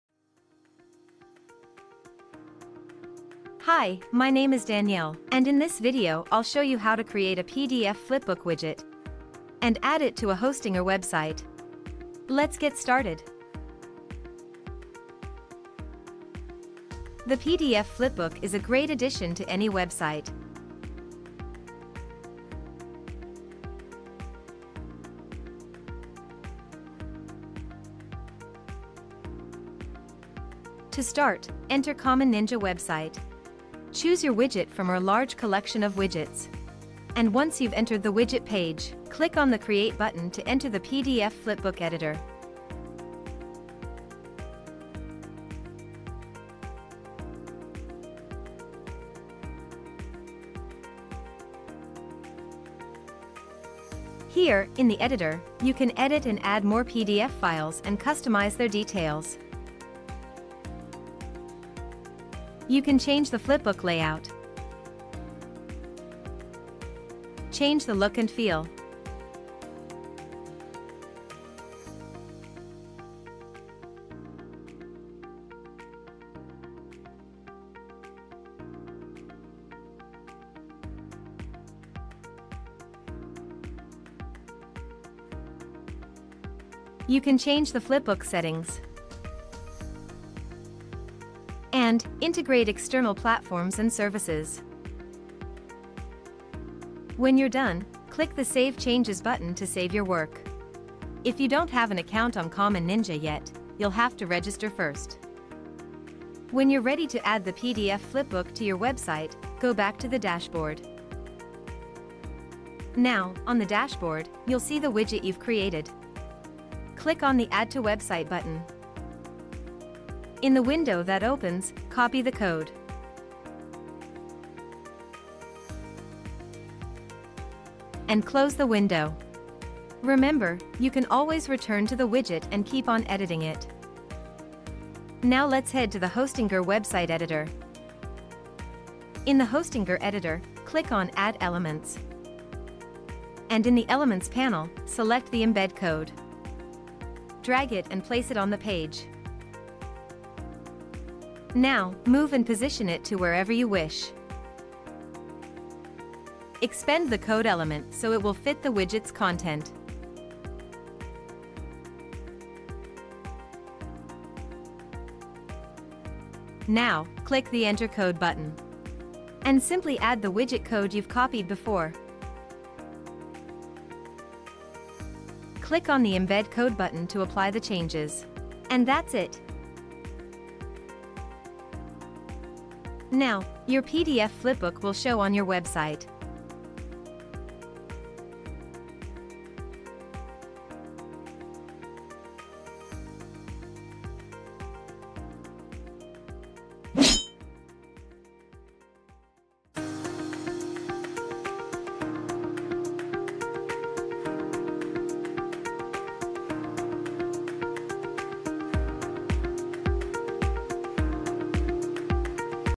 How to add a PDF Flipbook to Hostinger
Common Ninja's PDF Flipbook widget is a user-friendly tool that allows you to showcase your PDFs in a flipbook format directly on your website. This interactive format is a great way to engage your audience and present your content in a visually appealing way. The widget is fully customizable and mobile-friendly, ensuring a seamless experience for your visitors on any device.

Video Timeline
00:00 About the PDF Flipbook widget
00:30 Creating a PDF Flipbook
00:44 Editing the PDF Flipbook
02:15 Adding the PDF Flipbook to Hostinger

The PDF Flipbook widget for Hostinger has a wide variety of customizable features, including:
- Customizable Cover Image for Faster Loading
- Interactive Toolbar for Enhanced Navigation
- User-Friendly